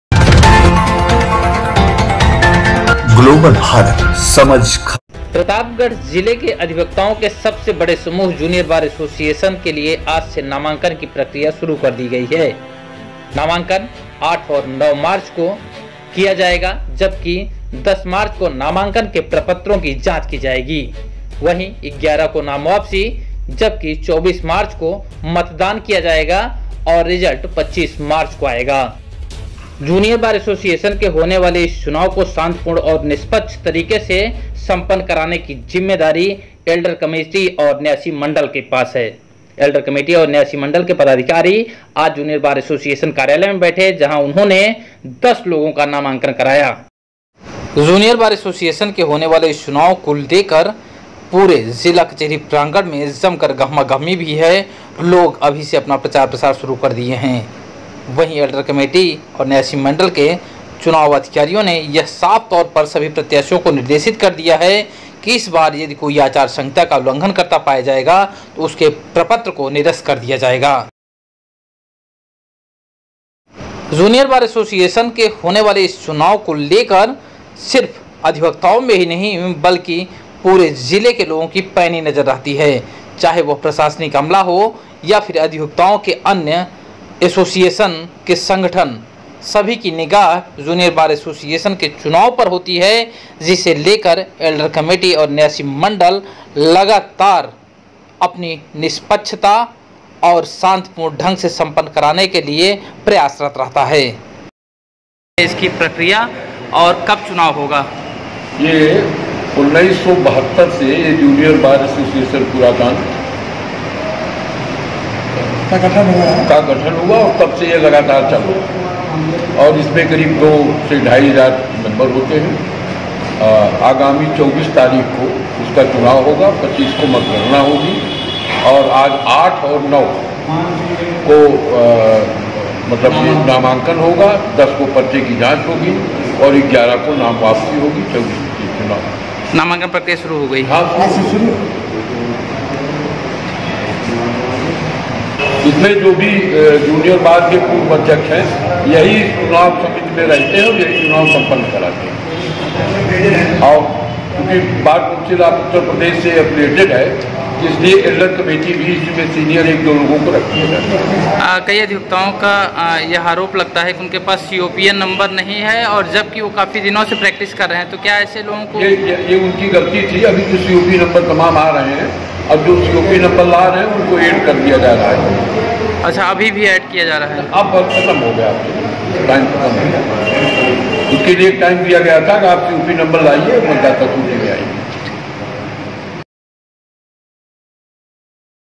प्रतापगढ़ जूनियर बार एसोसिएशन चुनाव 2021। चुनाव नामांकन और पूरी प्रक्रिया@GLOBALBHARATNEWS
global bharat news # hindi news channel Breking news Information news